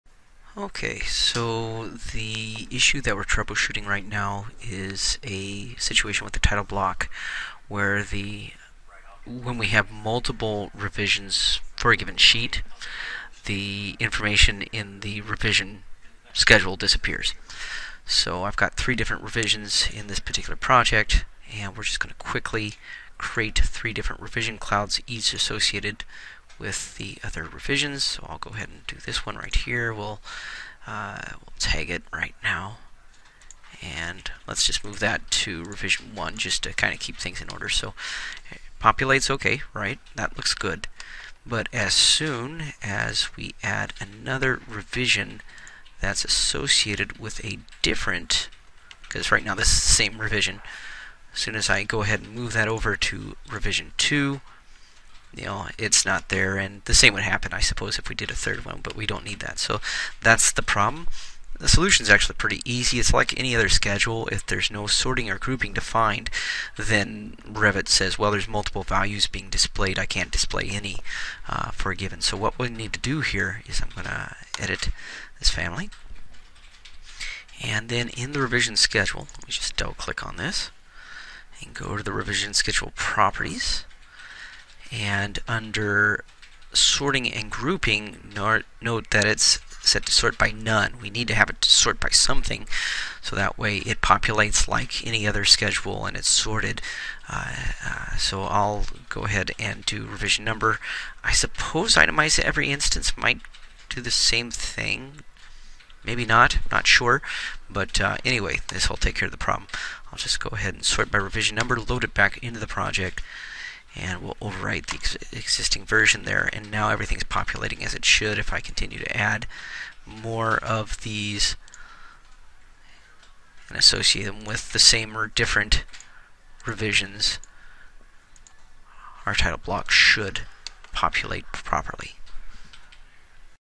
Troubleshooting Revision Schedules - Multiple revision problem
Revision schedules on title blocks in Revit are like other schedules--sorting needs to be defined in order for Revit to generate corresponding rows of data.  In this case, multiple revisions for a sheet would cause the revision schedule to go blank.  To fix it, we needed to apply sorting to the revision schedule inside of the title block.